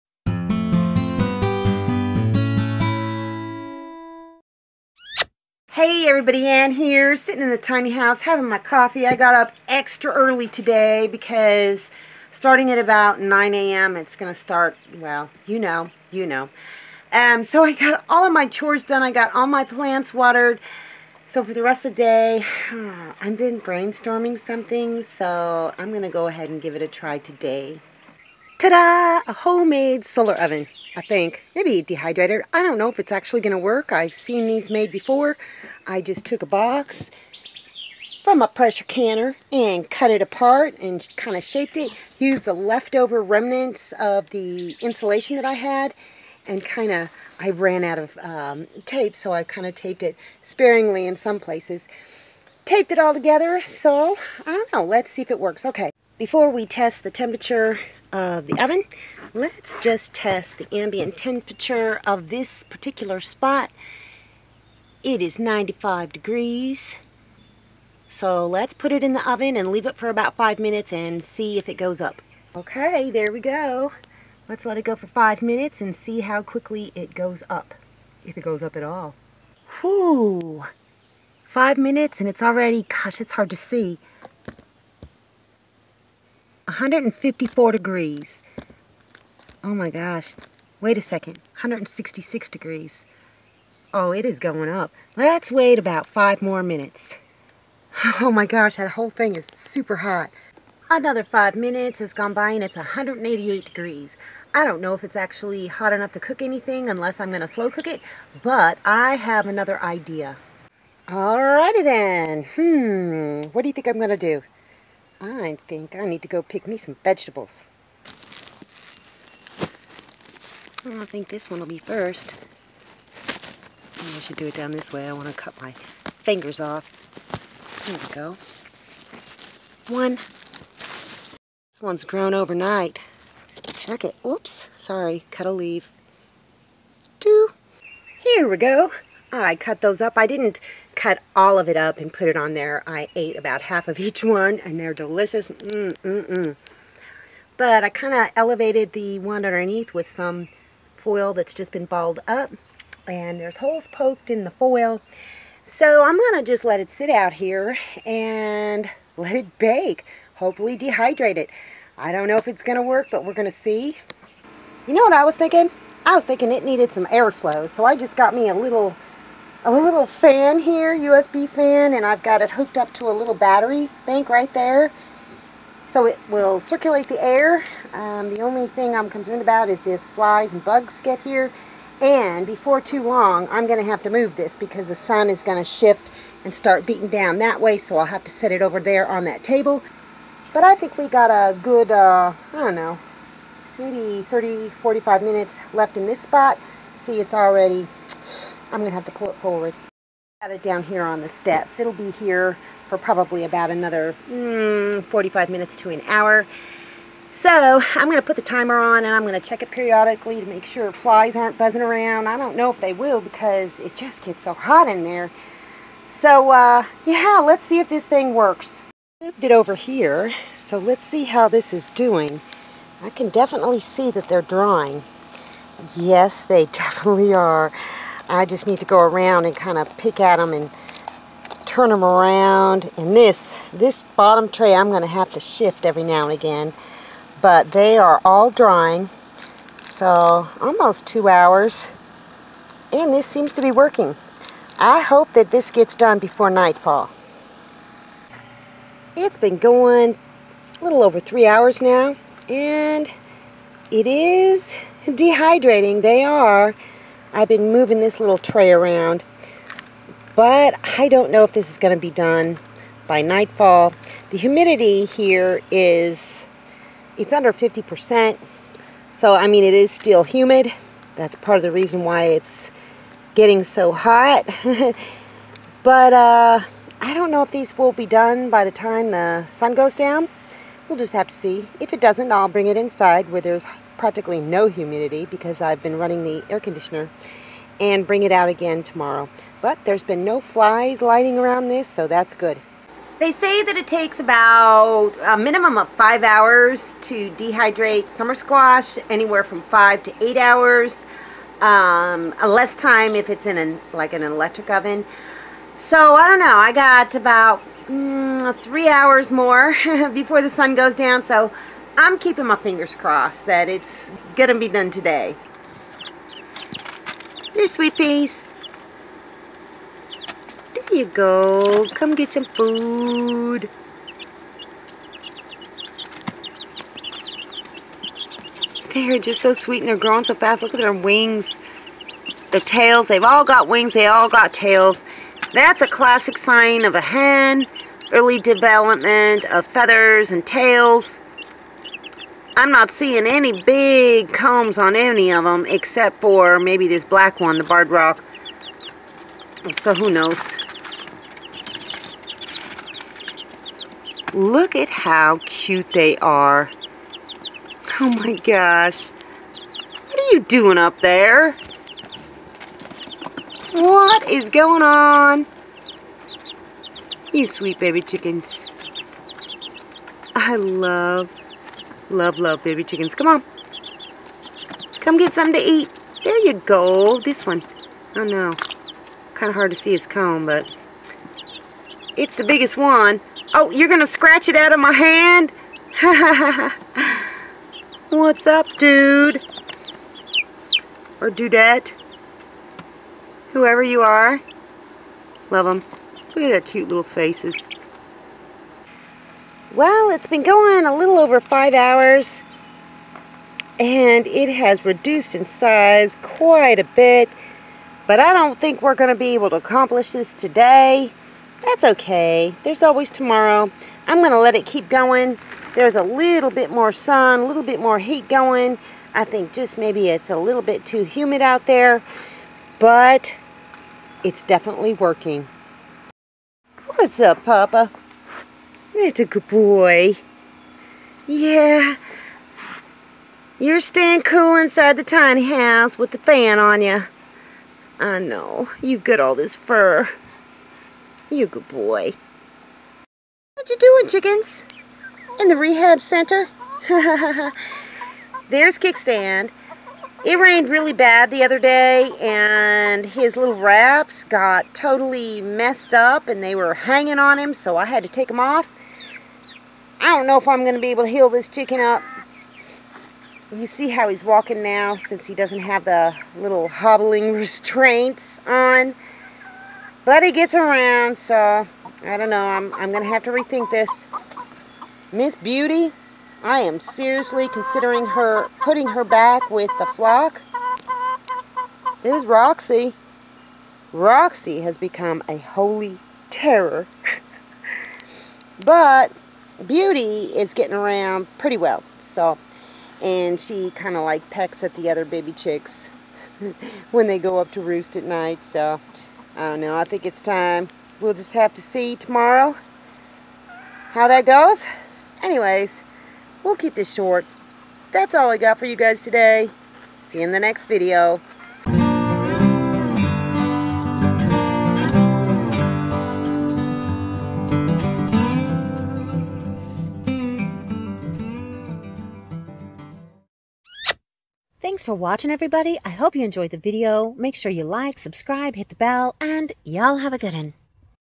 DIY Solar Cooker Dehydrator Made From a Cardboard Box - Ann's Tiny Life and Homestead
I felt the urge to make a thing so I made a solar cooker or dehydrator out of  a Cardboard box, some reflectix, and some foil tape. I got some veggies. Does it work?